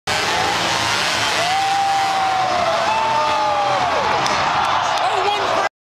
Supercross Indy 2009 - 4 Backflips - 1 Crash
Unfortunately this is only the start of the crash, but for those there, it will jog your memory.  After a small amount of time on the ground, the rider appeared to be completely recovered.